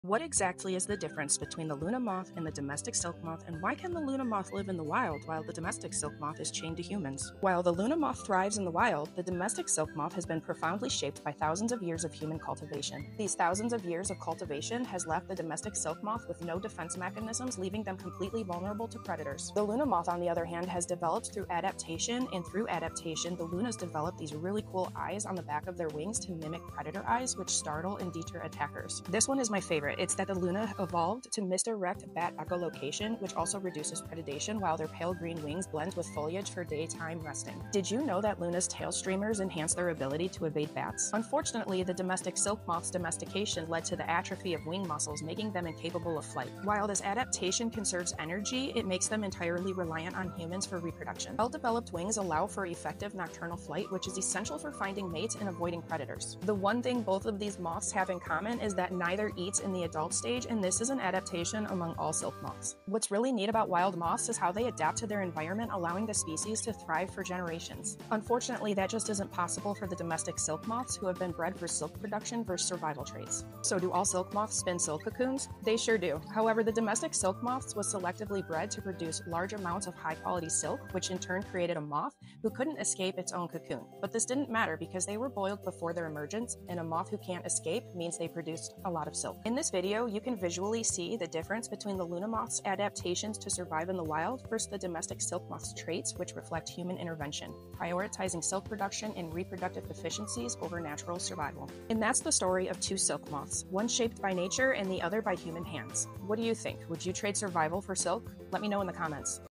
Evolutionary differences between the Luna and Domestic Silk Moth
What makes a moth wild vs domesticated?